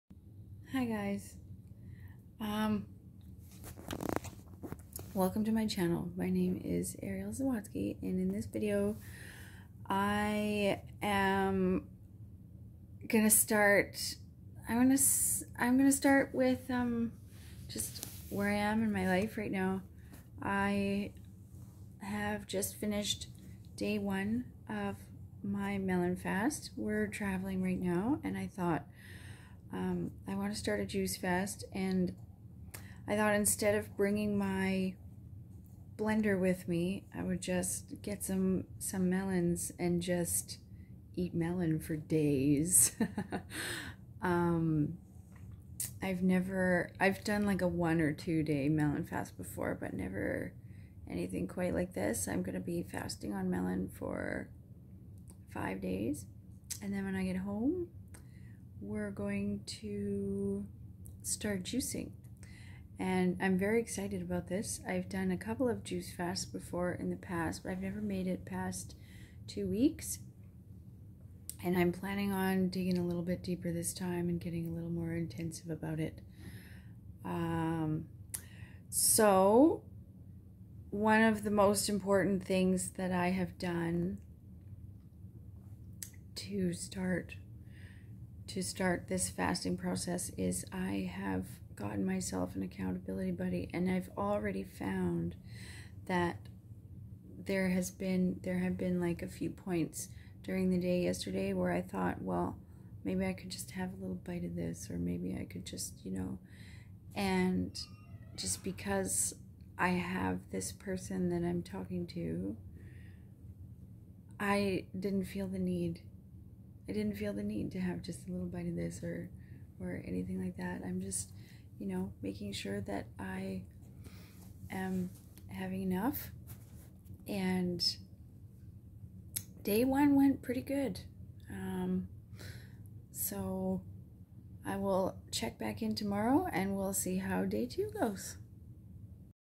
Day 1 of my melon fast
Rawmaste.
I just finished day 1 of my melon fast. It was made easy because I am not alone. I have a friend doing this with me. Even from the other side of the world.

Inspiration for the day. Together we can accomplish what we can not do alone ❤🙏